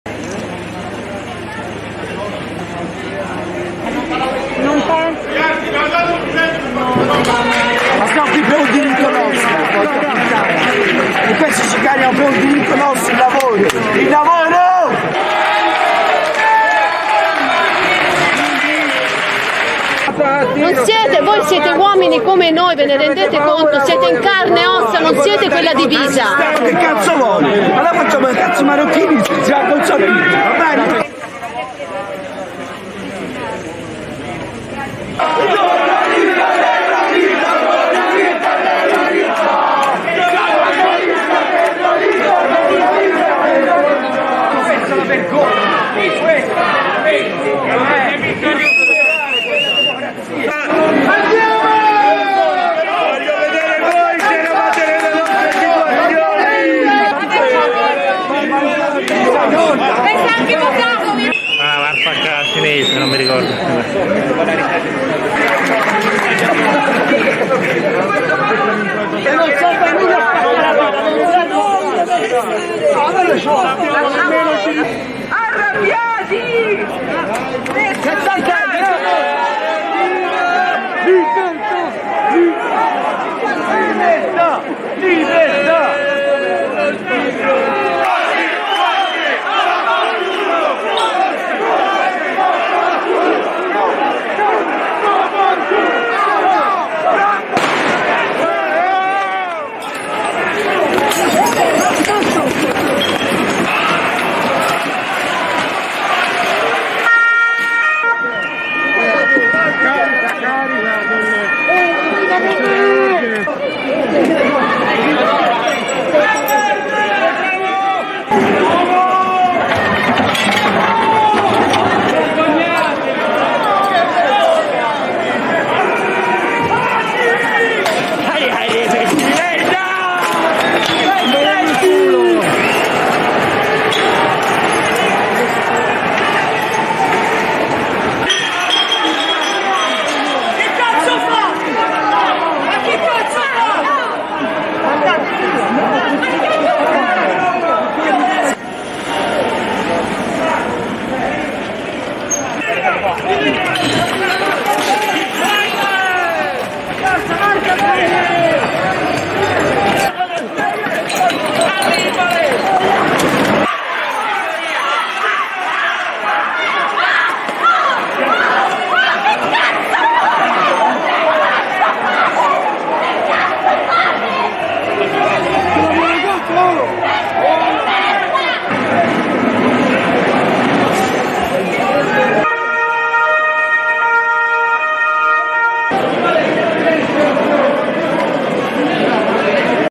Protesta contro il Dpcm a Firenze. Scontri con la polizia
In via dei Calzaiuoli i manifestanti hanno gridato slogan contro gli agenti e i giornalisti

CRONACA (Firenze). Proteste in piazza a Firenze contro il Dpcm, lanci di bottiglie contro la polizia. I manifestanti urlano slogan contro il governo, la polizia e i giornalisti. In via dei Calzaiuoli sono cominciati gli scontri, la polizia ha caricato i manifestanti che hanno provato a entrare in piazza della Signoria. Sin dai giorni scorsi il sindaco Dario Nardella aveva invitato la popolazione a non scendere in piazza per manifestare per non alzare il livello di tensione, la manifestazione non era autorizzata e non aveva colori politici. Lungo via dei Calzaiuoli gli agenti di polizia hanno cercato di disperdere i manifestanti che affollavano le vie del centro. La protesta non ha risparmiato l'arredo urbano usato nello scontro. Anche tra i manifestanti sono volate parole dure tra chi era sceso in piazza pacificamente e chi era lì per provocare. (e.delucia)